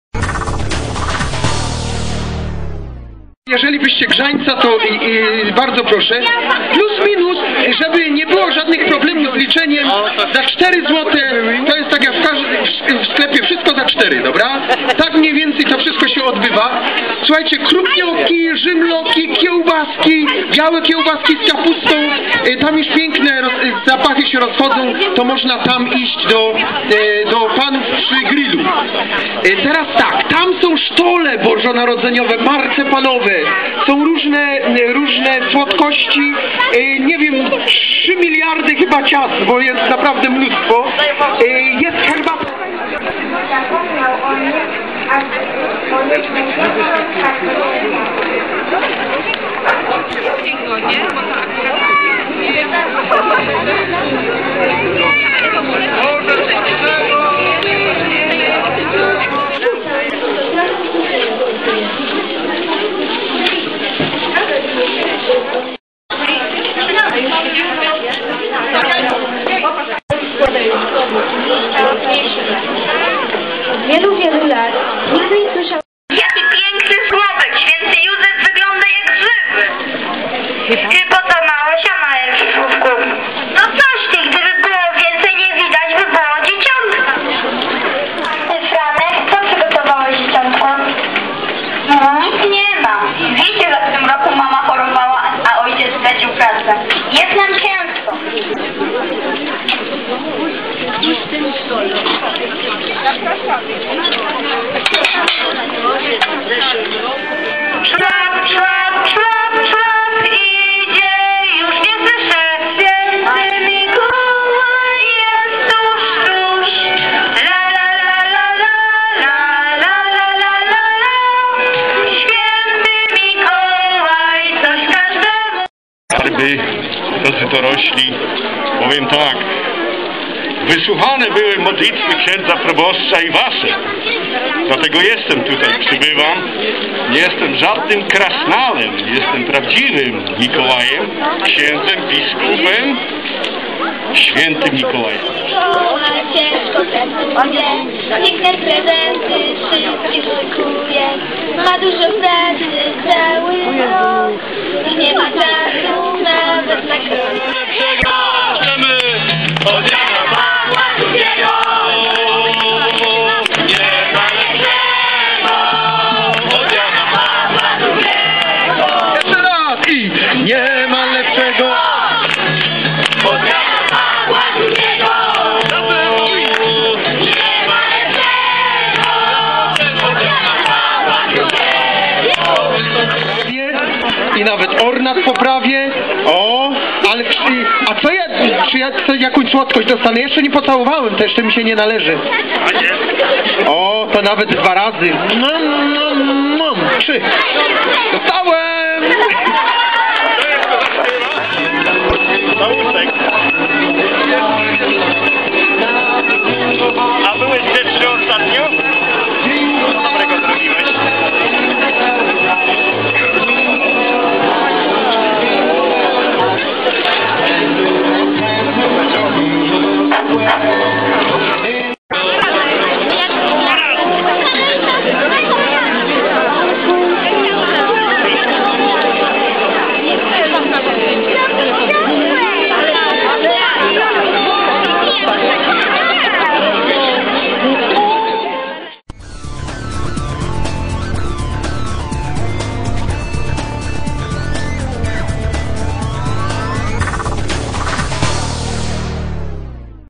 Mikołajkowy festyn przy parafii św. Marii Magdaleny
Prezentacja przebiegu festynu mikołajkowego, jaki 11 grudnia 2011 r. odbył się przy plebanii parafii św. Marii Magdaleny w Kuźni Raciborskiej.